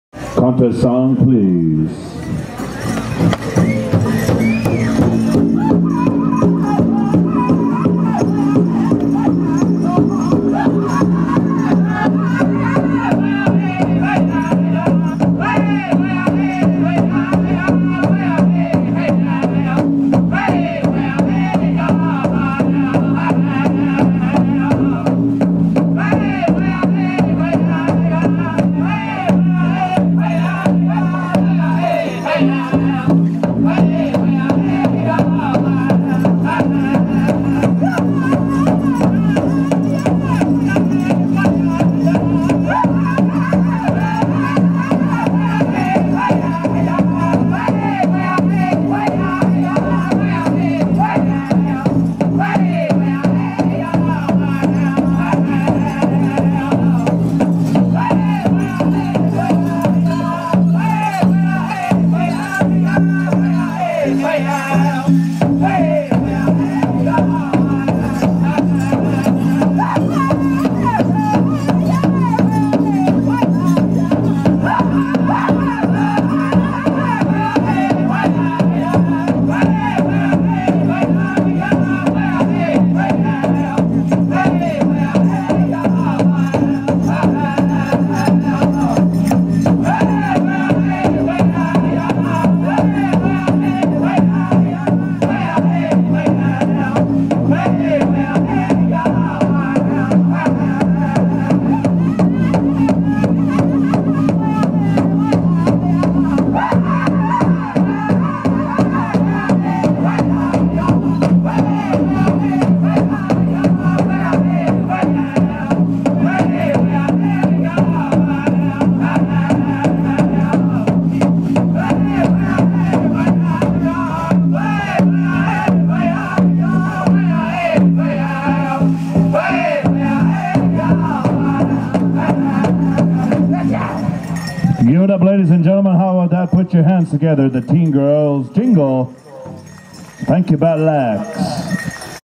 BATTLE AXE @ Julyamsh TEEN JINGLE DRESS
BATTLE AXE Singers at JULYAMSH Powwow 2013 on Sunday, song for TEEN JINGLE DRESS categpry, Couer D'Alene Tribe at Post Falls, Idaho